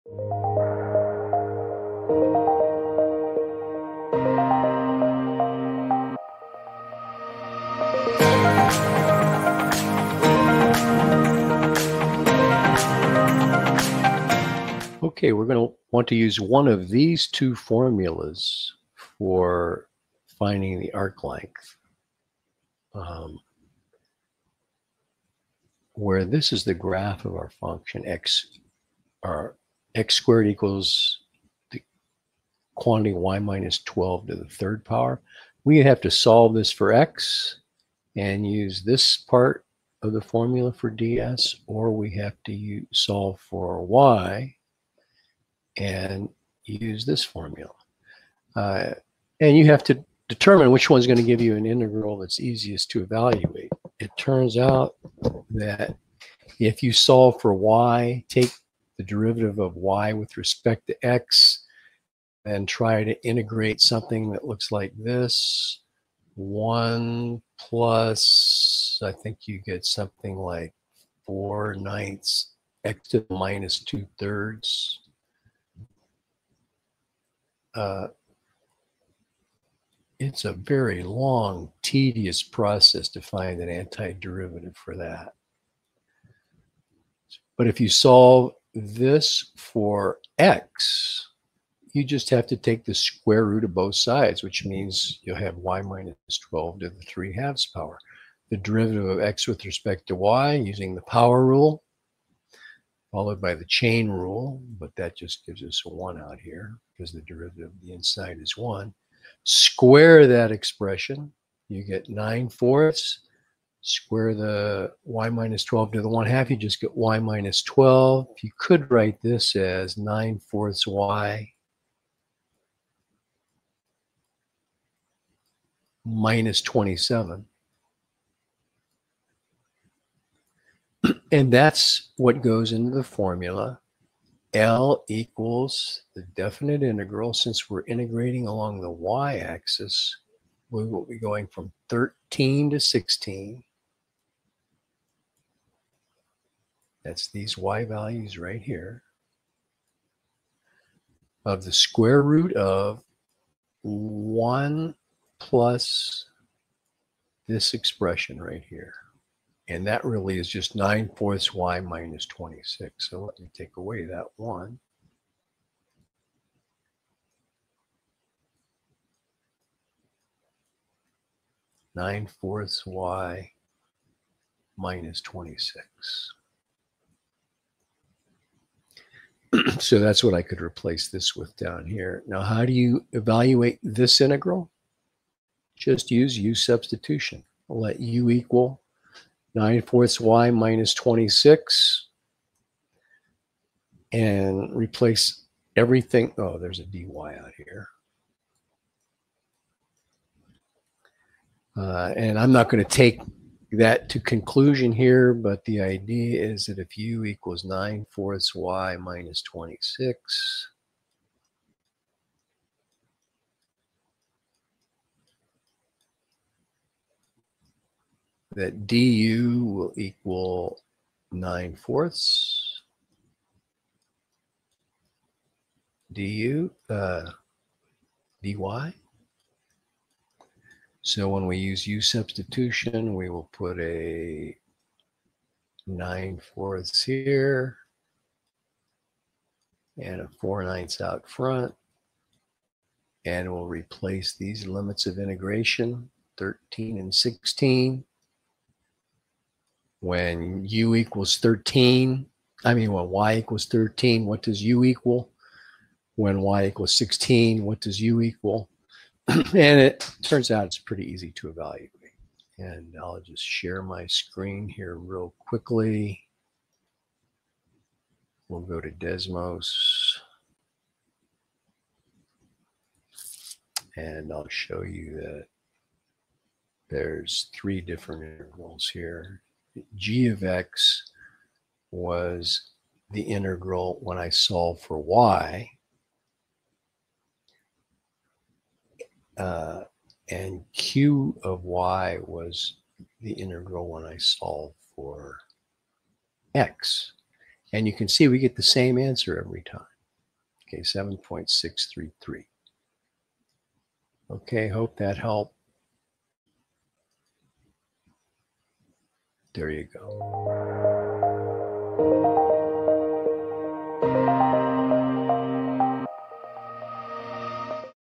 Find arc length given a choice of solving for x or solving for y
Question: Find the length of the arc of the curve from point P to point Q
x^2=(y-12)^3, P(1,13), Q(8,16))Please helpmy life depends on this

Answered By:

Doug C.